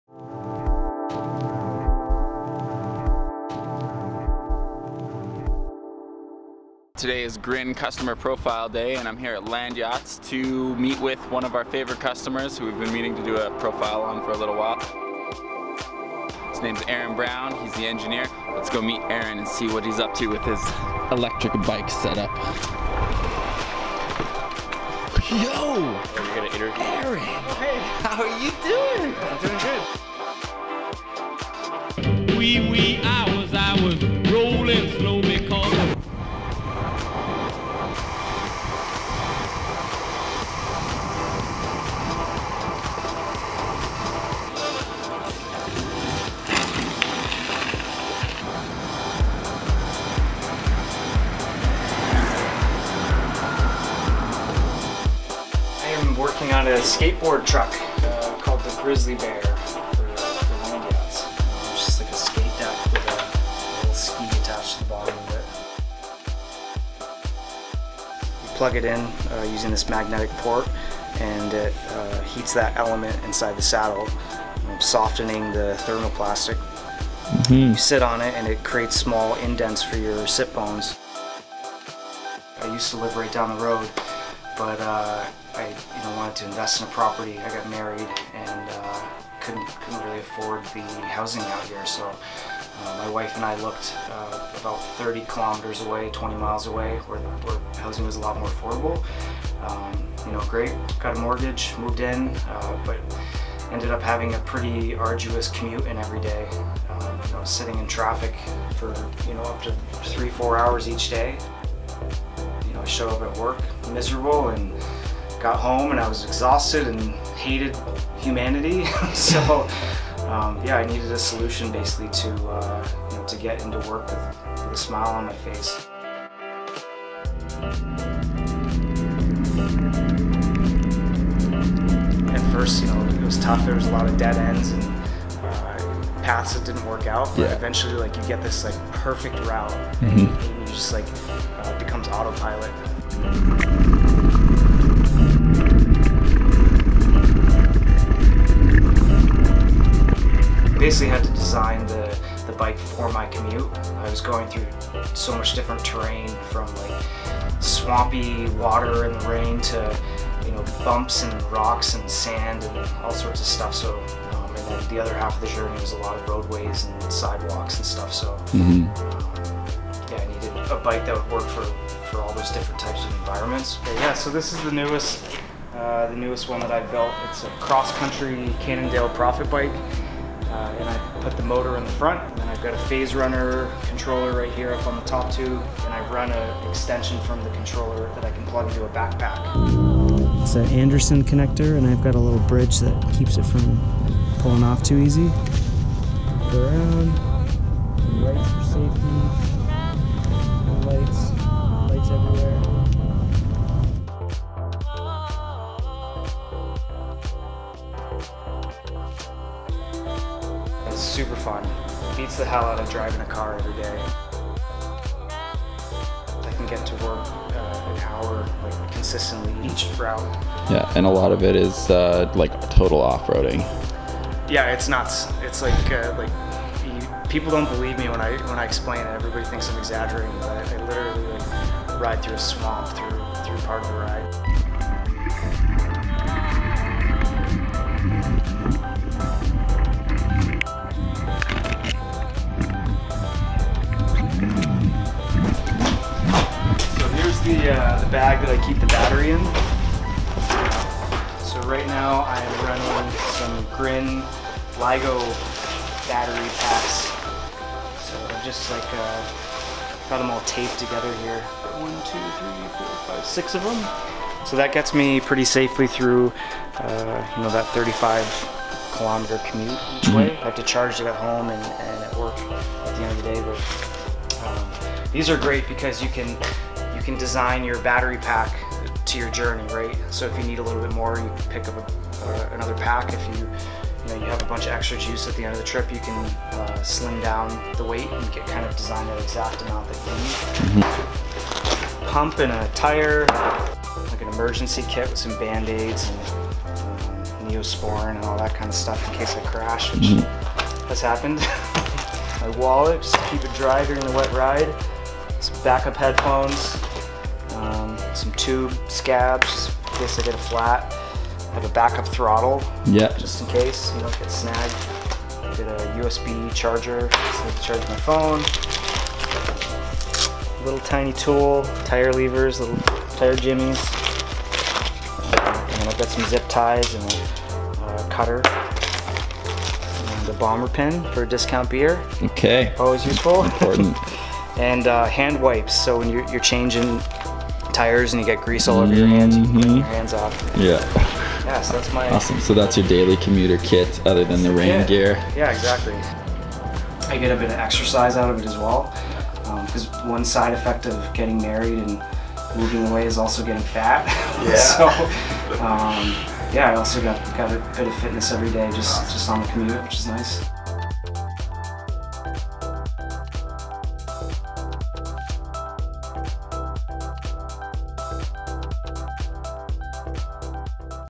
Grin Profile, Aaron Brown
In this video profile we have a look at Aaron Brown, one of the main engineers at the iconic Vancouver-base longboard company Landyachtz.  When Aaron first came to visit us many years ago we were excited because we thought he wanted to talk electric skateboards, but it turns out he was after an ebike kit like everyone else!

Aaron has gone through a few builds since then, often testing out the latest Grin components in the process. His most recent conversion was built to enable daily commuting from the suburbs to Vancouver without going crazy.  Rather than converting a conventional road bike and navigating through traffic, Aaron assembled a full suspension offroading machine and found a pathway to work that includes trails, bikelanes, swamps, roads, and everything in between.

This is hardcore ebike commuting at its best, and it shows how DIY electric bicycles can help creative millennials (like ourselves) cope with being driven completely out of Vancouver's real estate market.

Landyachtz is an awesome business here in Strathcona that has expanded from making longboards into other products like snowskates and even their own line of bicycles now.

For those asking about components used in the build, this using almost entire made-in-Canada components: